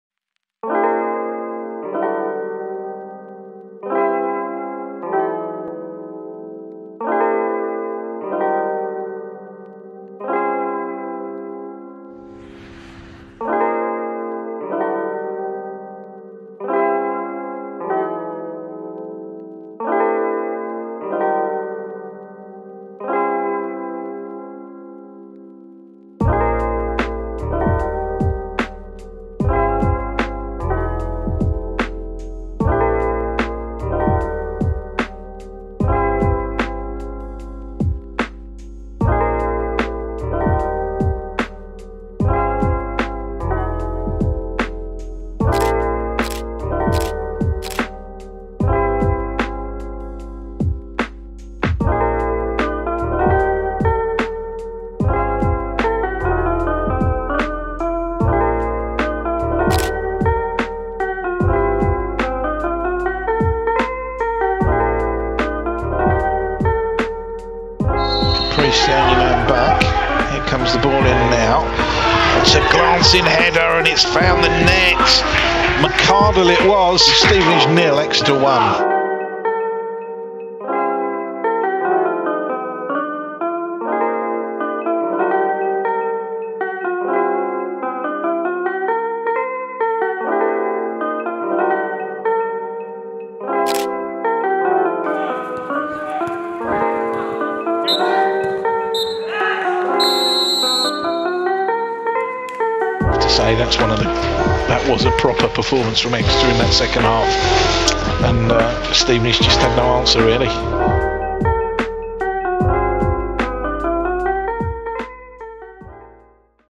🚗 On the Road: Stevenage | Exeter City Football Club
Caitlyn Ebsworthy made the trip to the Lamex Stadium on Tuesday night as City picked up 3️⃣ points